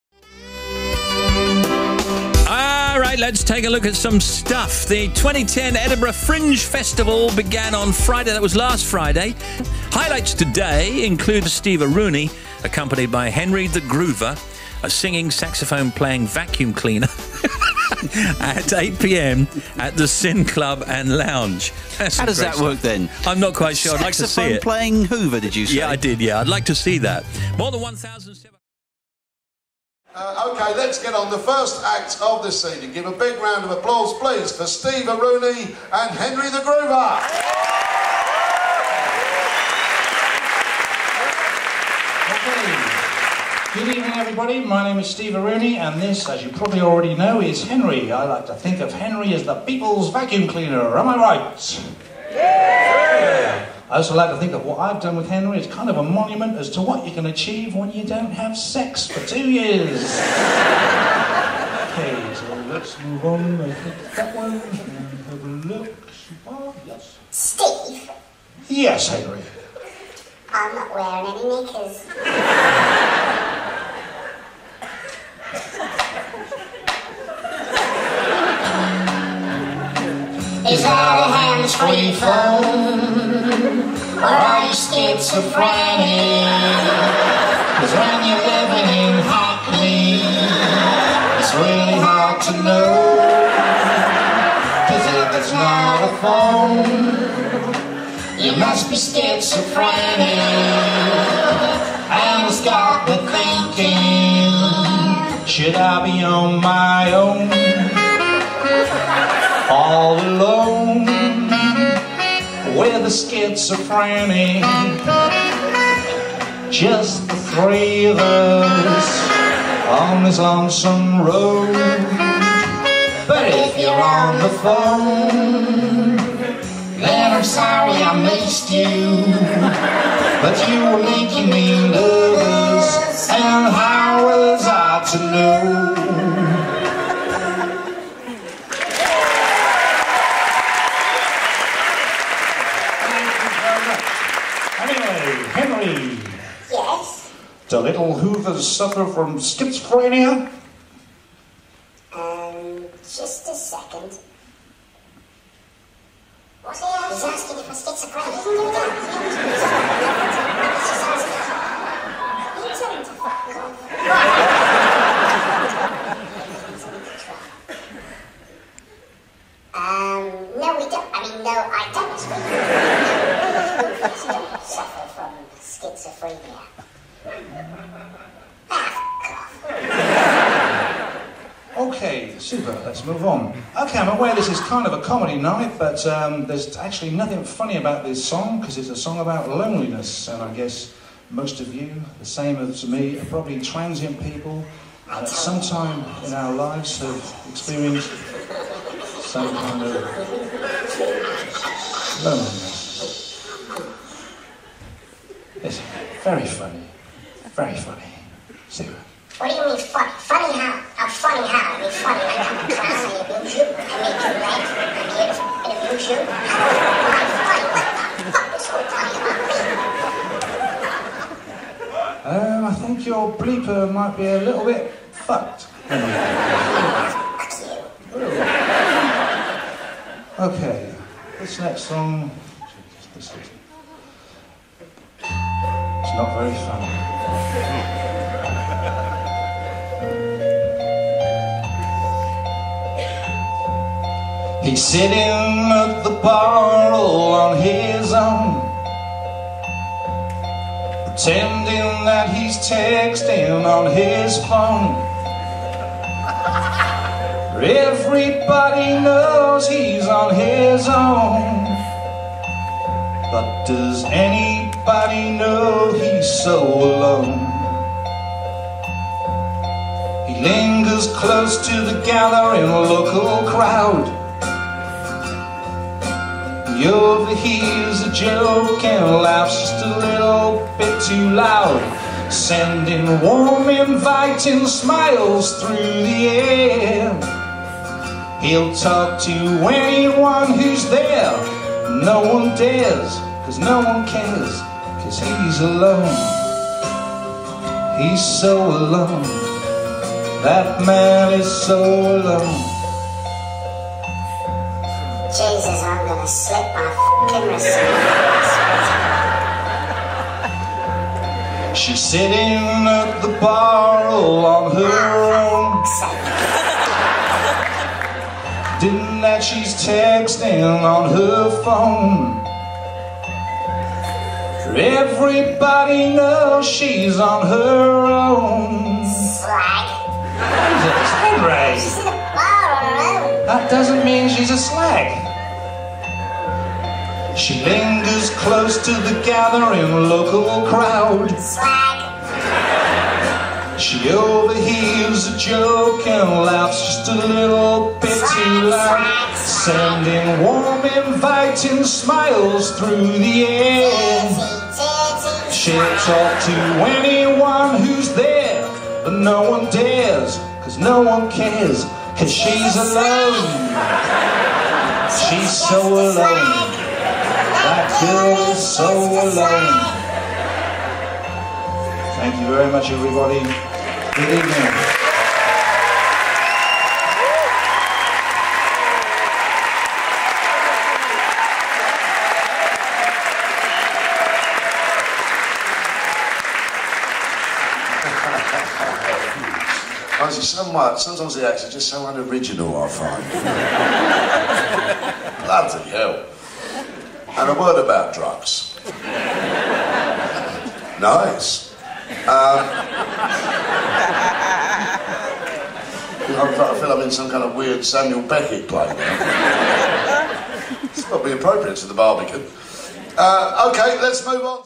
Steve Aruni and Henry the Groover Promo Vid
Steve Aruni and Henry the Groover Promotional Video of the Barbican (formerly at the Hackney Empire) New Act Of The Year Finals